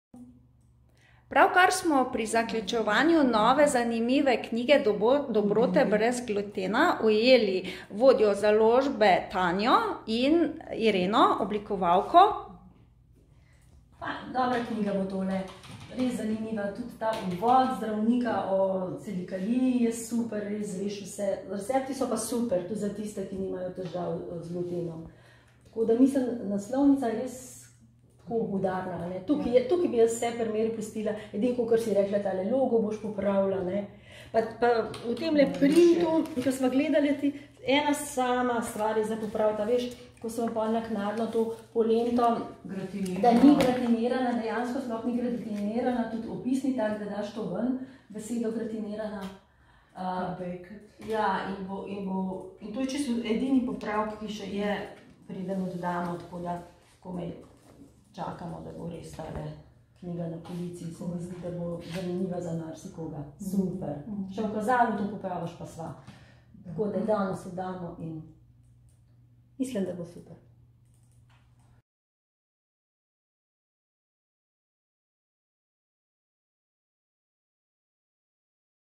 Nova knjiga DOBROTE BREZ GLUTENA že na poti v tiskarno
Vse več ljudi se sooča s kronično boleznijo tankega črevesa, ki je posledica preobčutljivosti na gluten oziroma t. i. celiakijo, ki zahteva strogo brezglutensko dieto. Dosledno upoštevanje diete bolnikov na srečo ne omejuje pri pripravi okusnih jedi. Navodila za pripravo odslej najdete tudi v novi knjigi, ki vključuje recepte za različne vrste kruha, peciva, sladic, prigrizkov, glavnih jedi in prilog. Brezglutenske jedi, ki so jih avtorji dvojezične, slovensko-angleške knjige oblikovali v sodelovanju z IC Piramida Maribor, je v fotografski objektiv estetsko ujel priznani mojster fotografije Tomo Jeseničnik. Predstavljene jedi vsebujejo le naravne, brezglutenske sestavine, kot so koruza, kvinoja, riž, ajda in proso. Pomemben del vsebine knjige je tudi prispevek zdravnika Jerneja Dolinška, ki nas poduči o celiakiji, njenih znakih in posledicah ter nas osvesti, da tovrstne bolezni ne smemo spregledati oziroma zanemariti.

Blanka Vombergar, Jernej Dolinšek, Ivan Kreft, Marija Horvat, Stanko Vorih
DOBROTE BREZ GLUTENA / GLUTEN-FREE DELICACIES
140 strani, dvojezično besedilo, trda vezava, 21 × 21 cm, cena 16 EUR